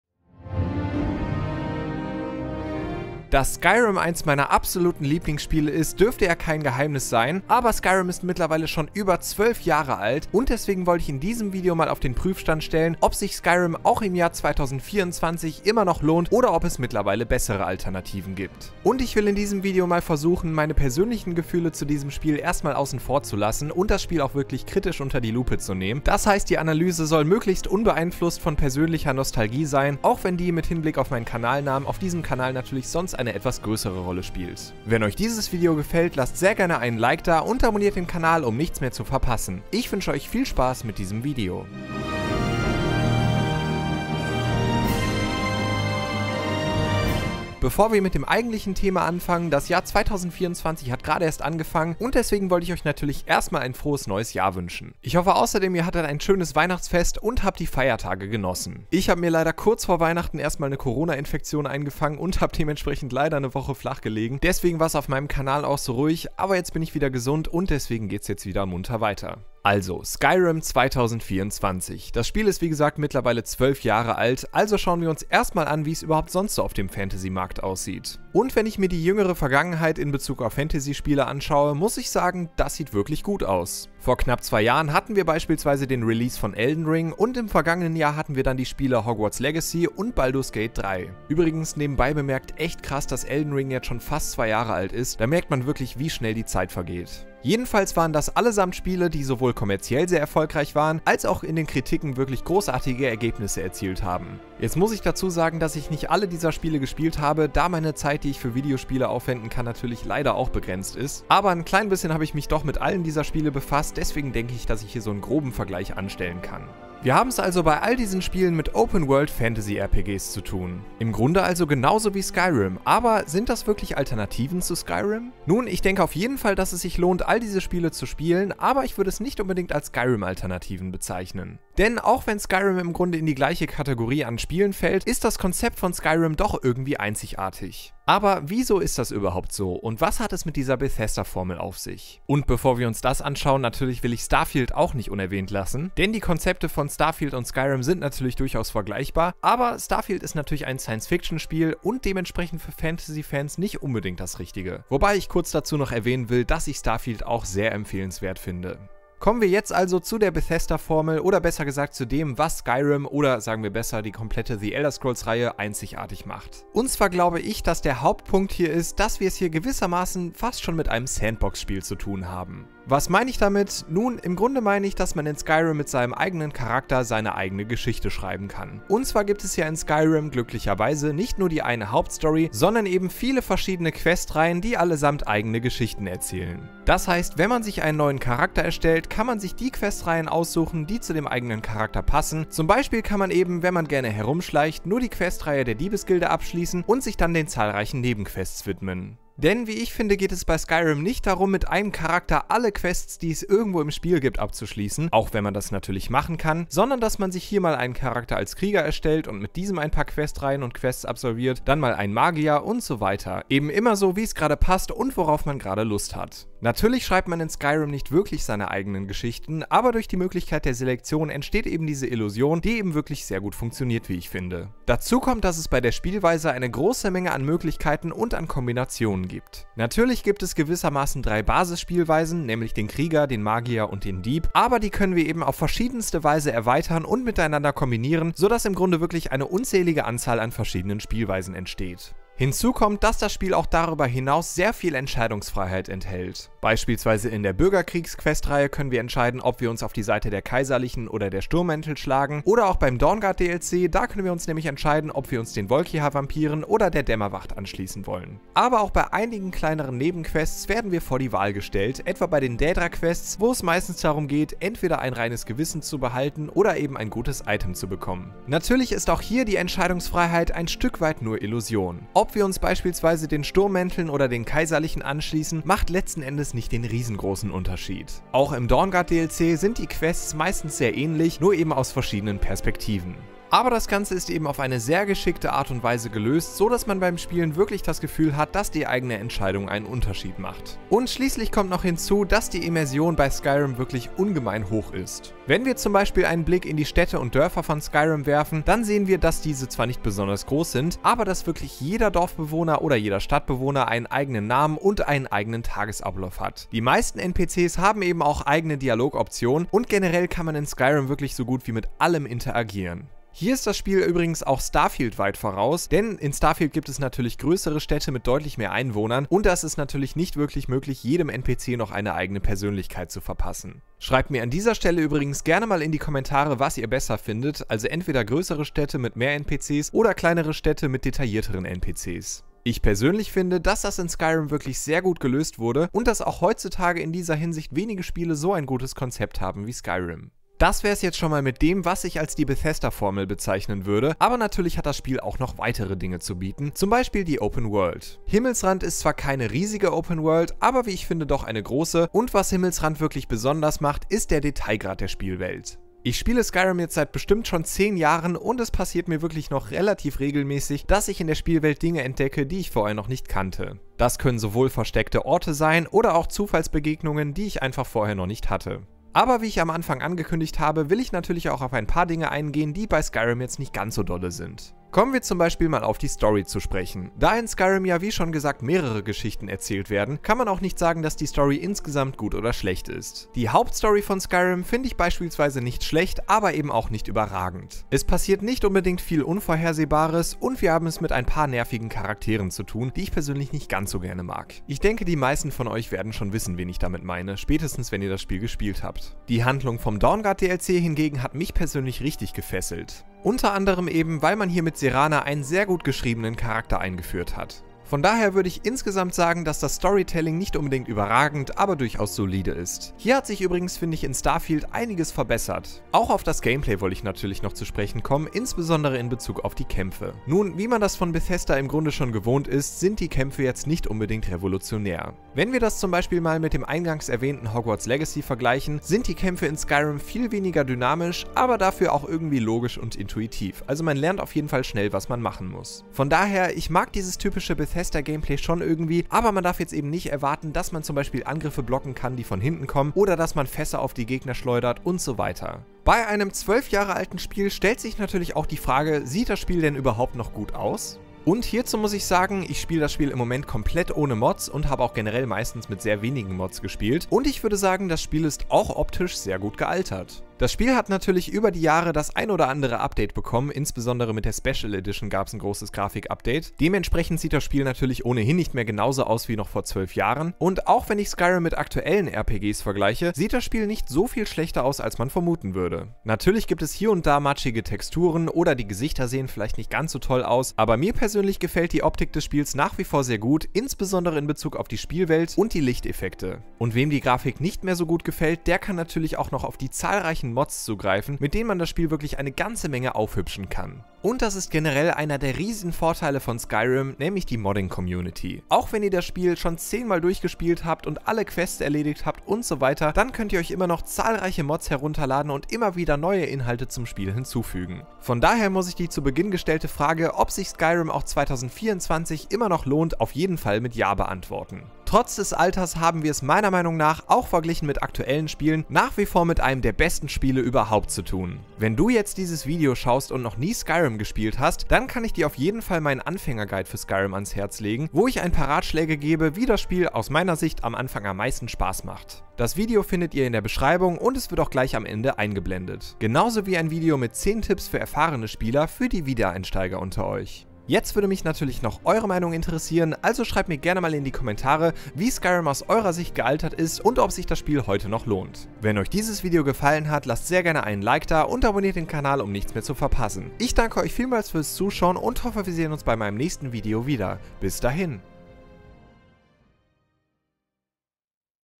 12 Jahre Skyrim: Lohnt es sich 2024? | Review
The Elder Scrolls 5: Skyrim ist mittlerweile schon über 12 Jahre alt! Seitdem sind natürlich zahlreiche gute Fantasy-Rollenspiele erschienen, daher stellt sich die Frage: Lohnt sich Skyrim auch 2024 noch, oder gibt es inzwischen bessere Alternativen? Dieser Frage wollen wir in diesem Video nachgehen. Viel Spaß!

Du interessierst dich für gute RPGs & Spieleklassiker und möchtest keine neuen Videos mehr verpassen?